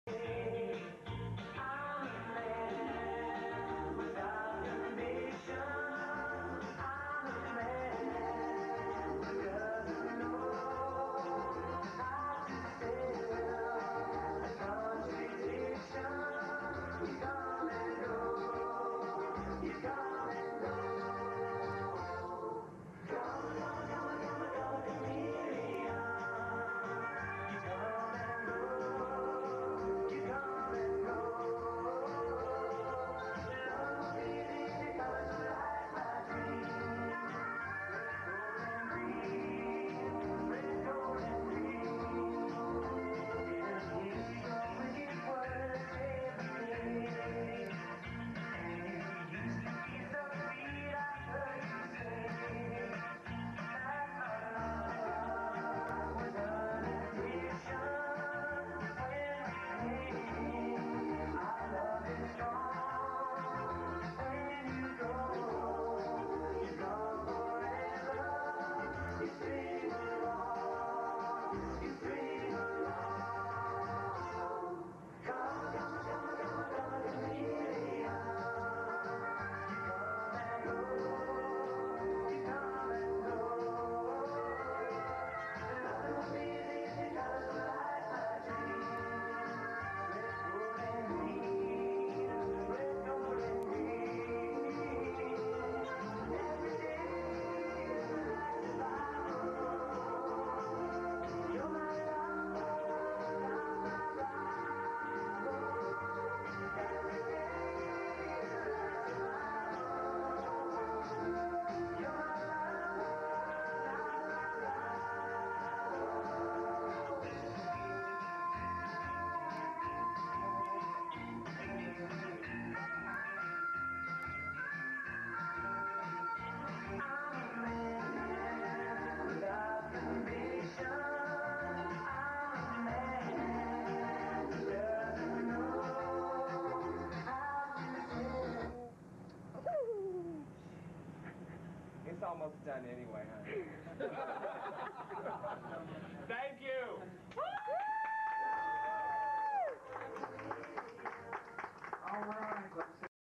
Diannah Nacole
Diannah Nacole performing at Lola's in Tulsa,OK on Sept 10,1995 - - I always thought Diannah did an amazing job with this performance-- wow!